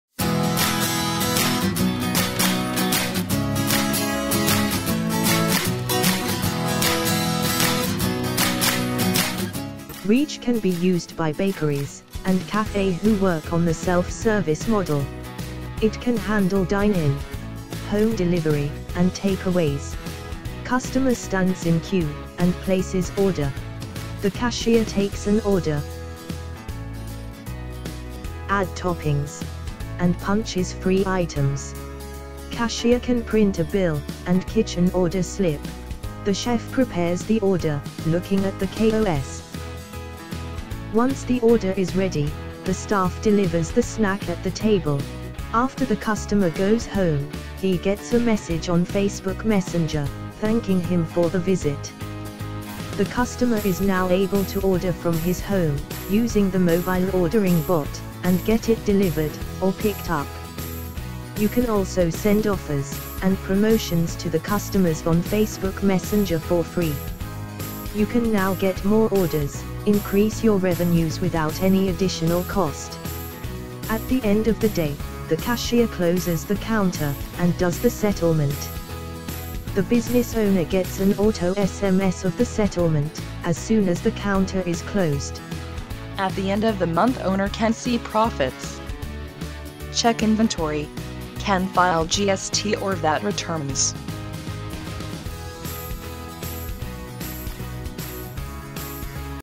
Billing Software Demo for CAFE, BAKERY AND RESTAURANT
Reach is a ERP Software for cafe, restaurants and bakery with a attached ordering chatbot which does the work of a server/sales executive. It also helps you to send promotions to your customers.